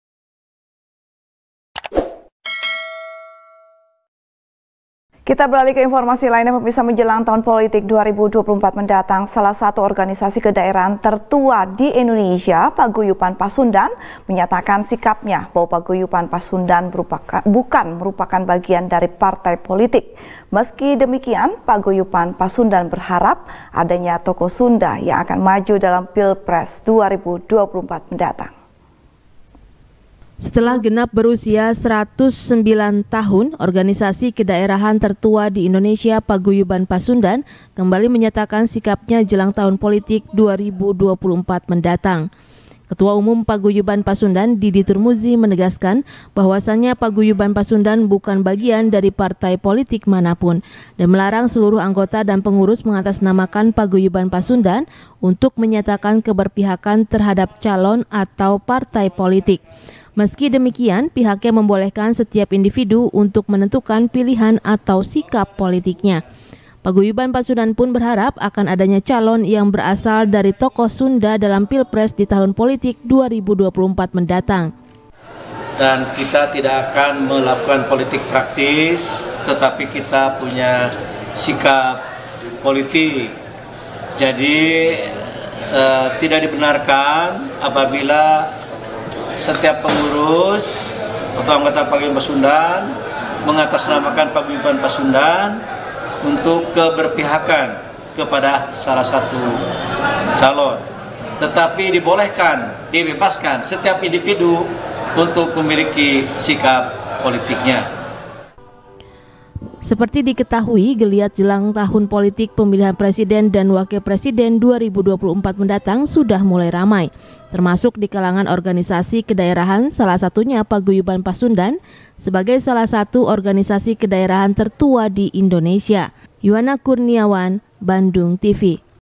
Paguyuban Pasundan Bukan Bagian Dari Partai Politik Manapun | SBR | BANDUNGTV
SBR | BANDUNGTV | FARHAT MUMTAZ | Jelang Tahun Politik 2024 Mendatang, Salah Satu Organisasi Kedaerahan Tertua Di Indonesia, Paguyuban Pasundan Menyatakan Sikapnya, Bahwa Paguyuban Pasundan Bukan Merupakan Bagian Partai Politik. Meski Demikian, Paguyuban Pasundan Berharap Adanya Tokoh Sunda, Yang Akan Maju Dalam Pilpres 2024 Mendatang.